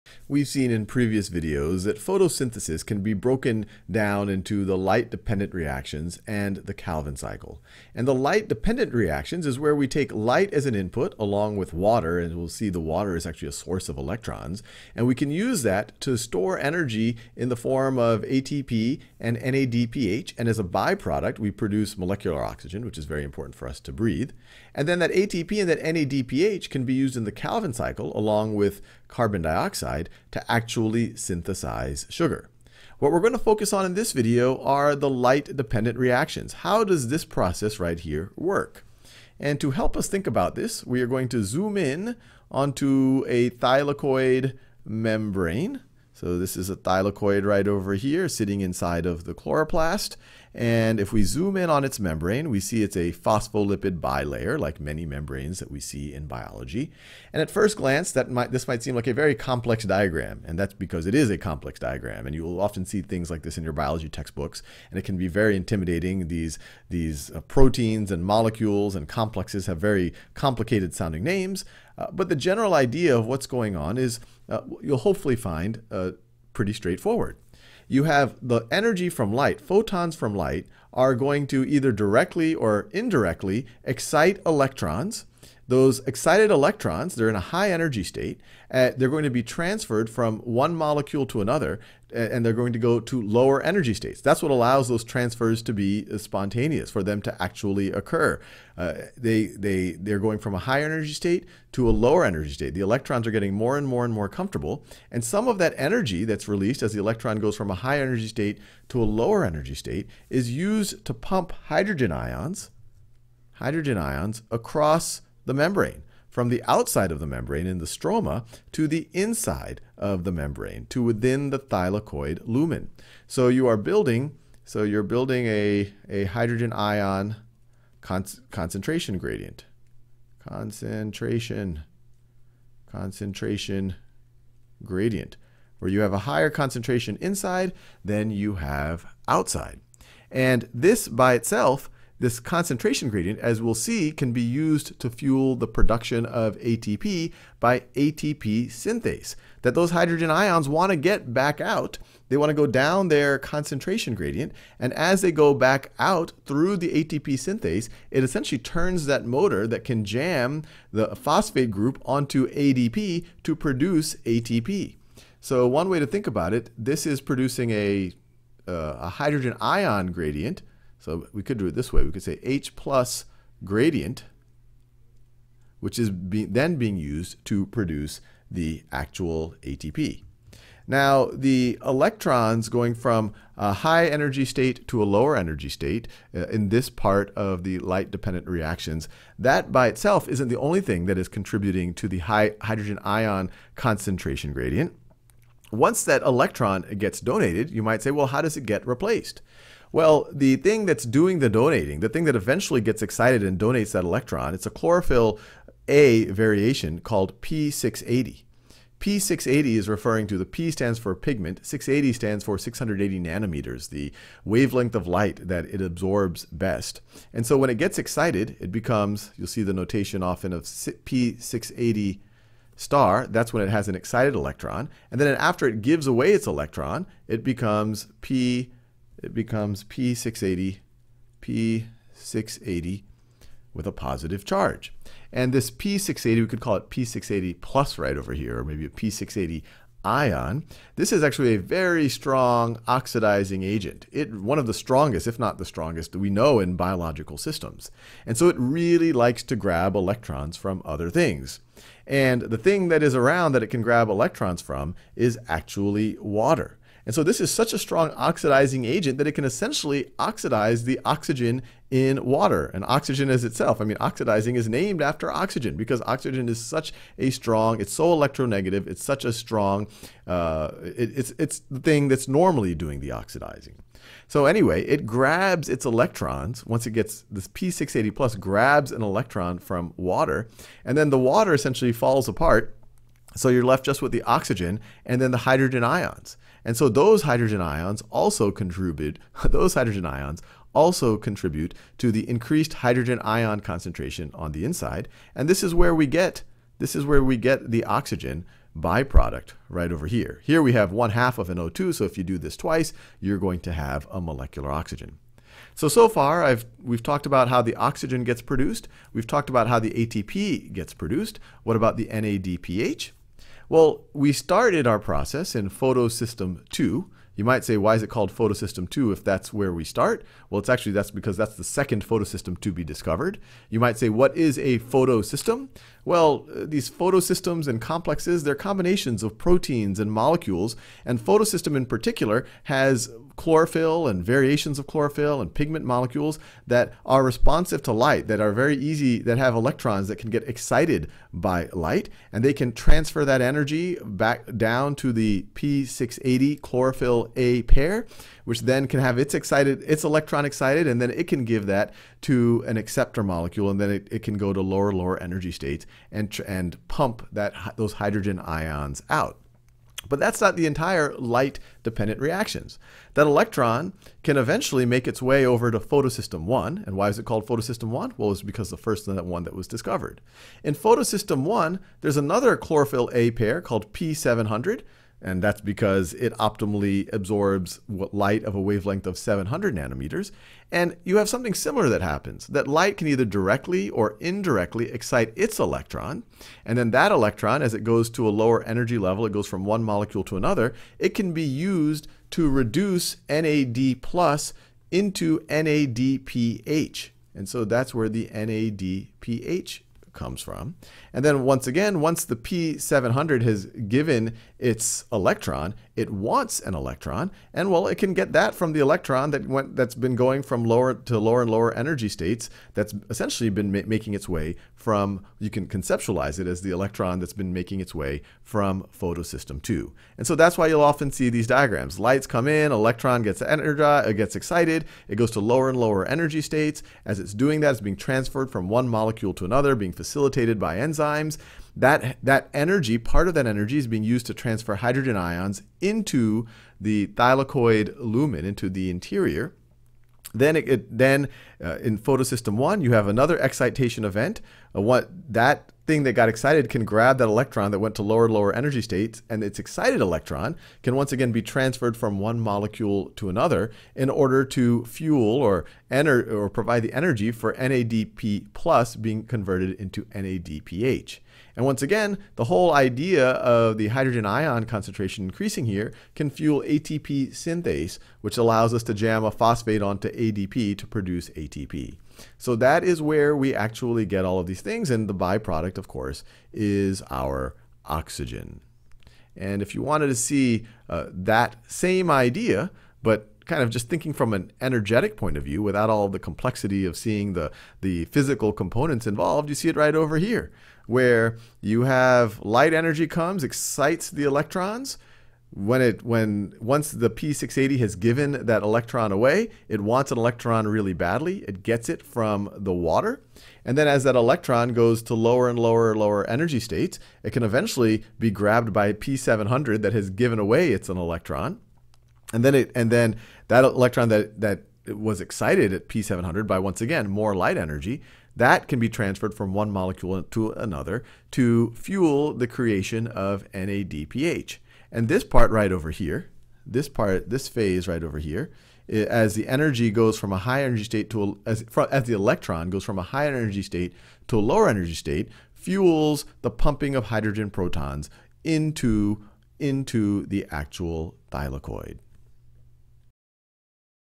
Conceptual overview of light dependent reactions
Courses on Khan Academy are always 100% free. Start practicing—and saving your progress—now

Seeing how light energy can excite electrons which can be used to create ATP and NADPH (with oxygen as a byproduct).